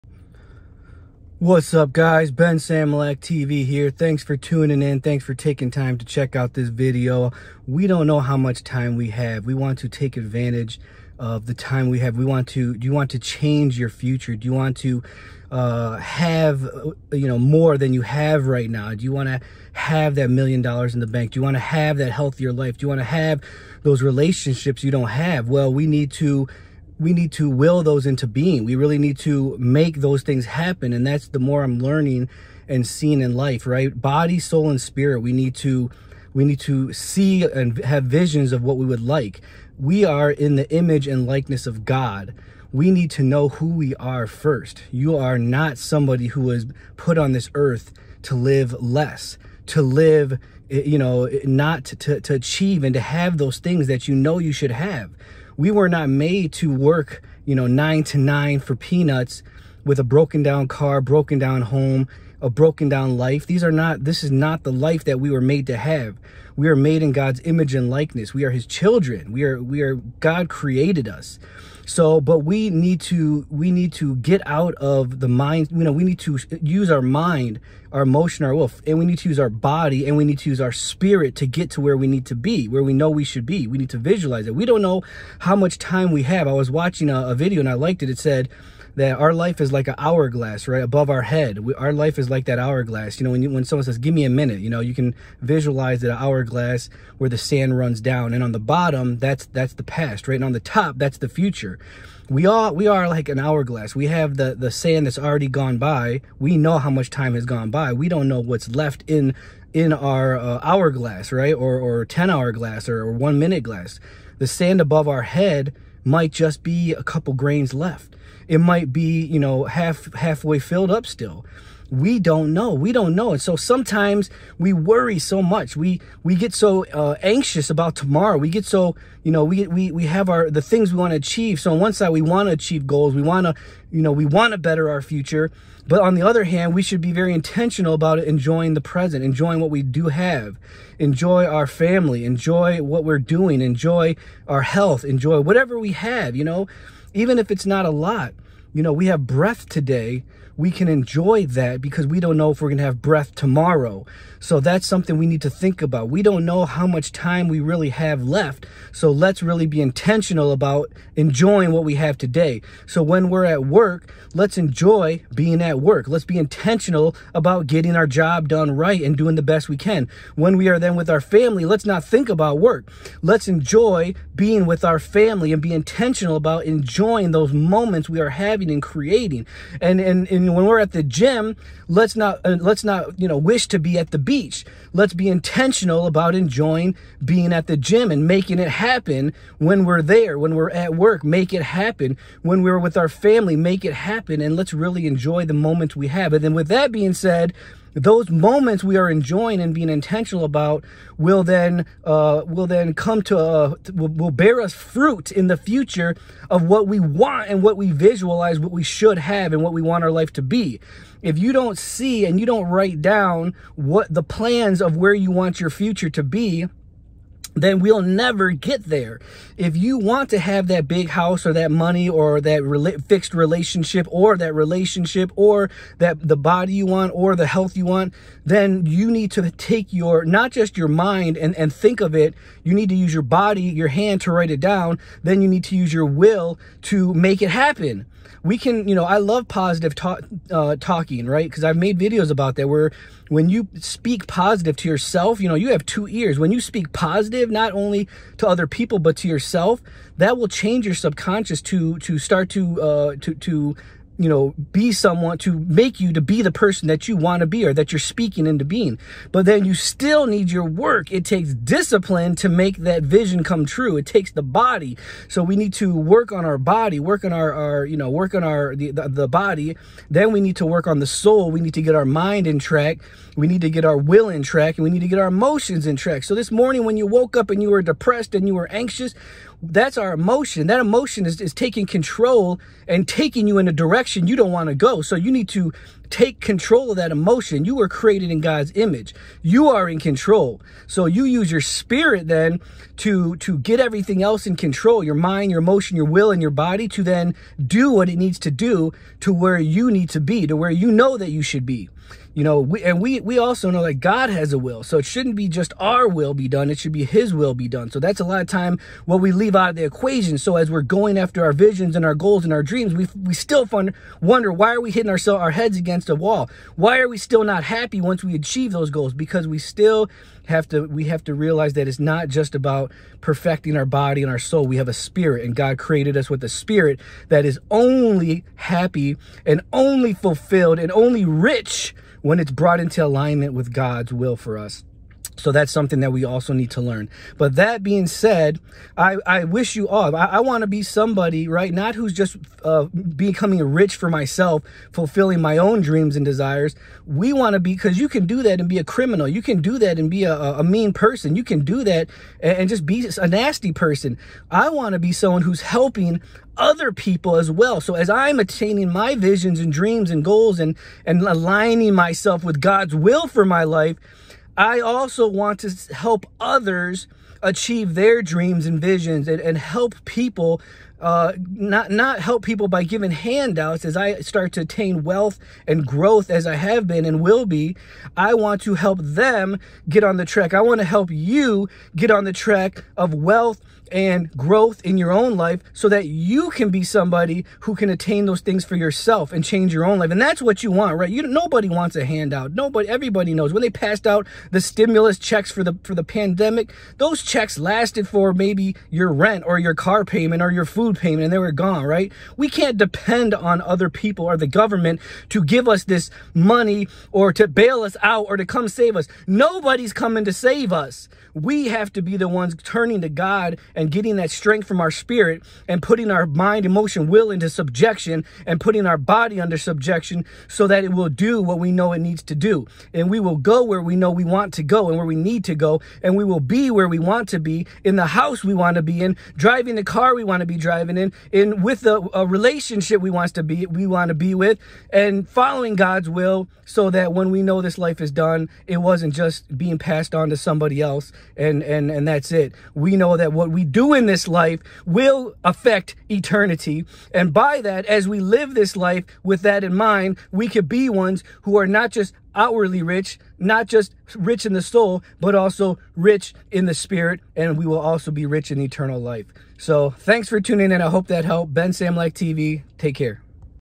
You are Created In God's Image
Welcome to BenSamulakTV. Today we discuss why people are not living to their full potential.  Many people go through life working for a small amount, living in a home they don't like, driving the car they don't want, and looking for a relationship that will make them happy.  You can have more, you should have more. Don't settle for less. Make the changes now to create and bring the future you want into being. Make sure to like and sub and hope this video brought you some light and pointed you to a brighter future. Have a great week!!